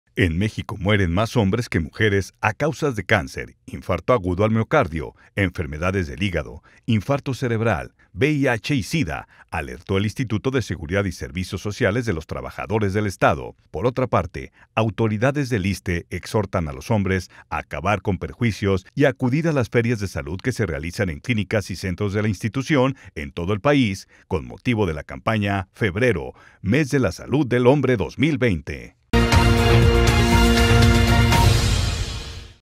Hombres, bajo la lupa
En México mueren más hombres que mujeres a causa del cáncer, infarto agudo al miocardio, enfermedades del hígado, infarto cerebral, VIH y Sida, alertó el Instituto de Seguridad y Servicios Sociales de los Trabajadores del Estado (ISSSTE).

Por otra parte, autoridades del ISSSTE exhortaron a los hombres acabar con prejuicios y acudir a las ferias de salud que se realizan en clínicas y centros del instituto en todo el país con motivo de la campaña “Febrero, mes de la salud del hombre 2020”.